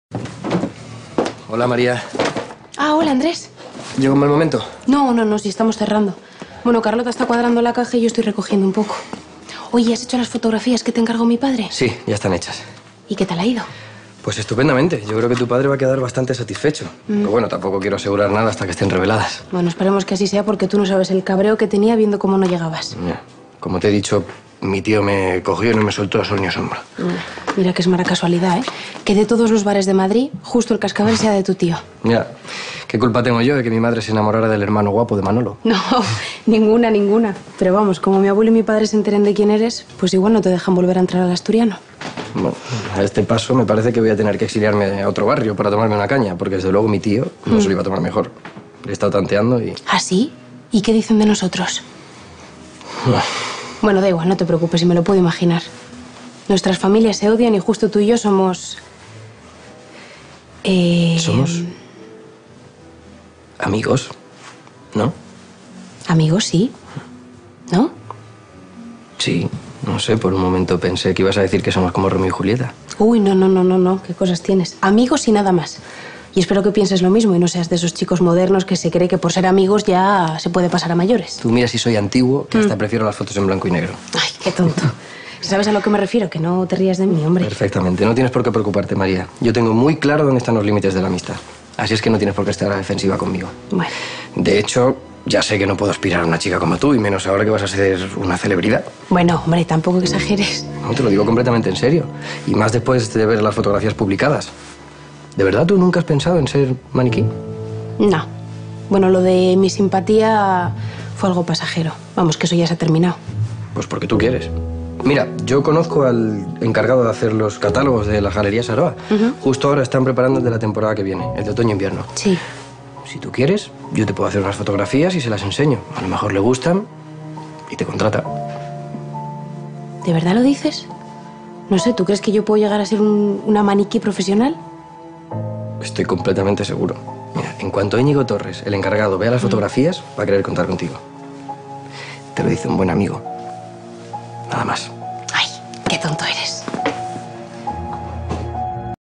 ¿Con qué ayudará Andrés a María?
Después de haber visto la maravillosa entrevista de María en el periódico y lo guapa que aparece en las fotos, Andrés decide hacerle una propuesta. María acepta a hacerse unas fotos para enseñárselas a un buen amigo de Andrés para la próxima temporada otoño-invierno.